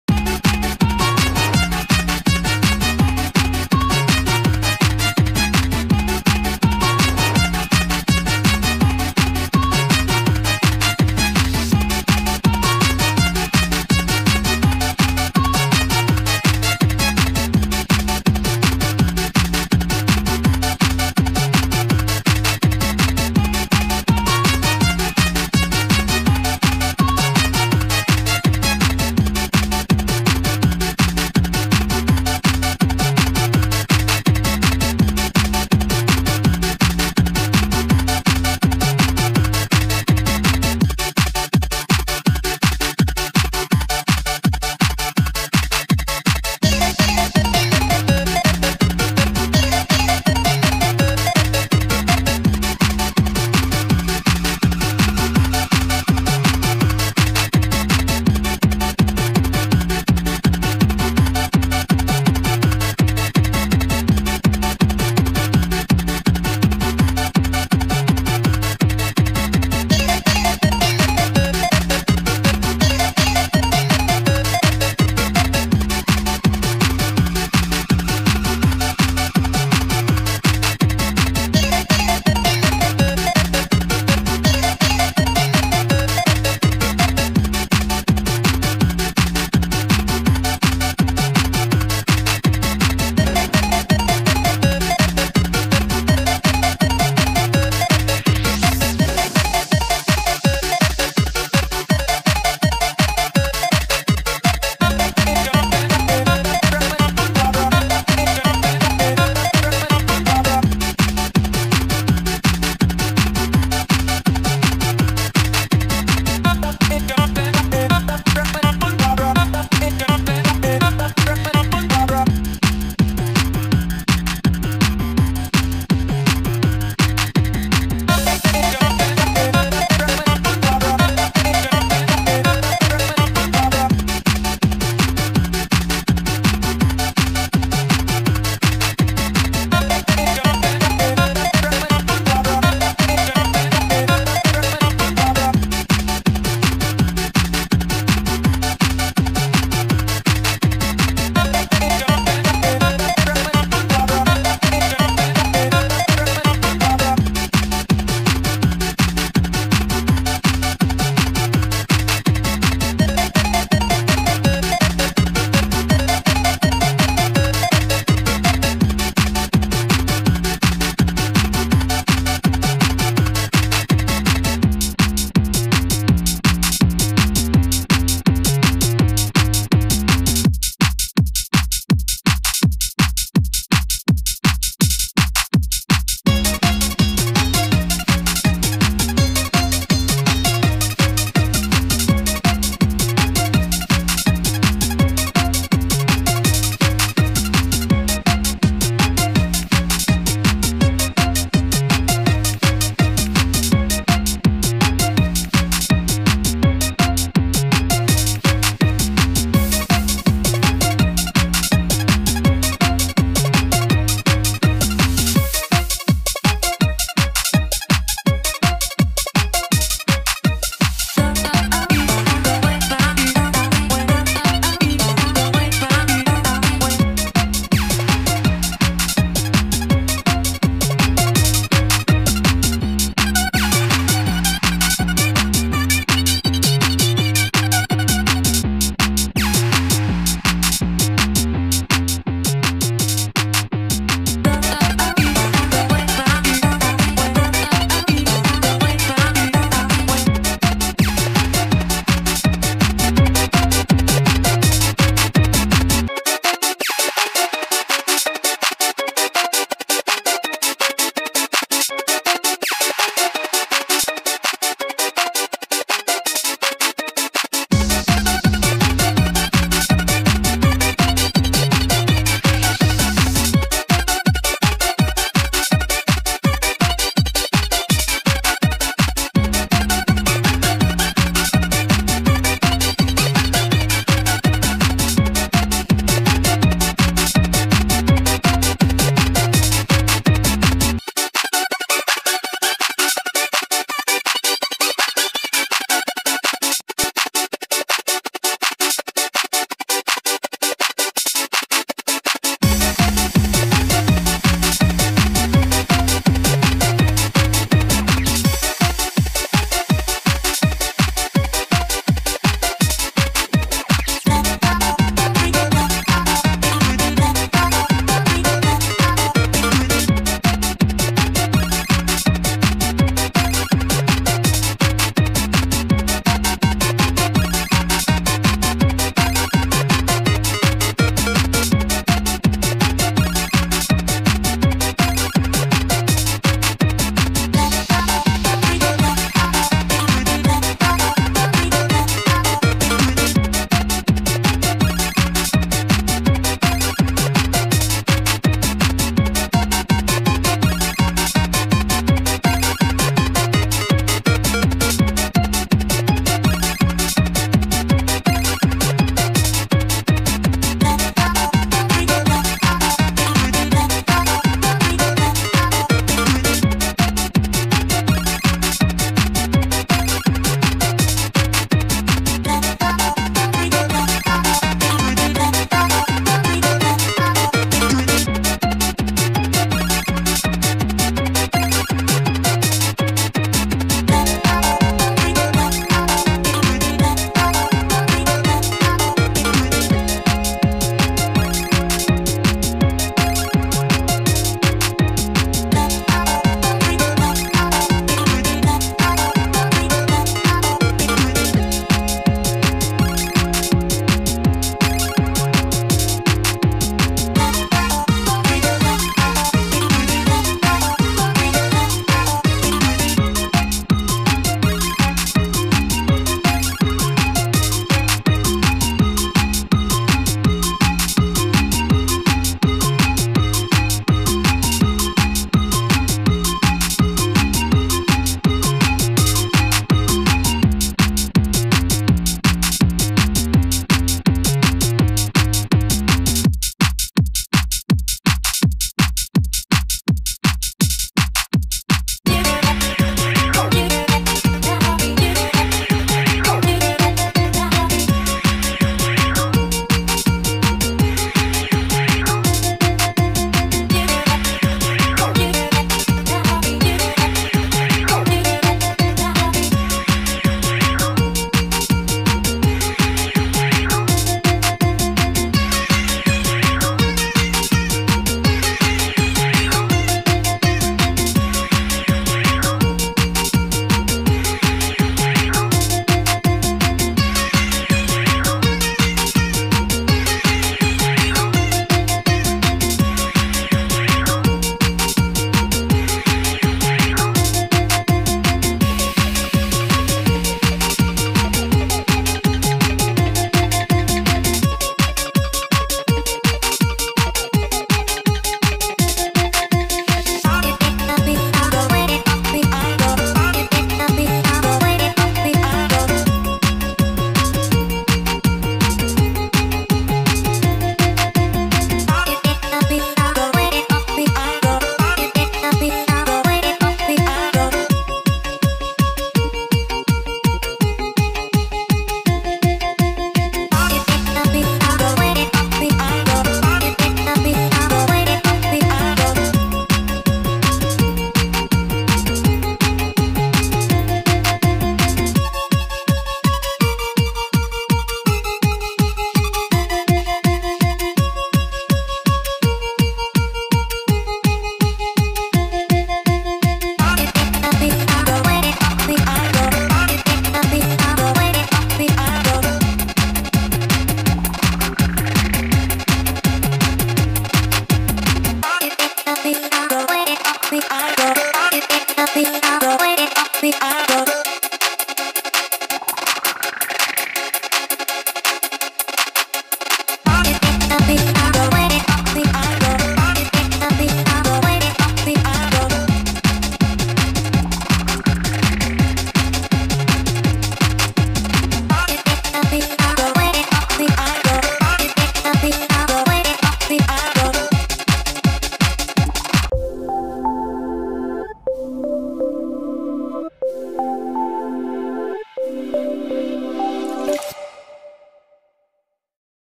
10 Minute Upbeat Music Lofi Timer 🕒 | Bunny Bliss 🐰🌼
Take a delightful 10-minute break with our Lofi Timer featuring adorable bunnies amidst the sunny dandelions of California. Let the whimsical ambiance and soothing tunes transport you to a world of relaxation and creativity. Perfect for a quick moment of rejuvenation or a brief study session, this video promises a refreshing escape.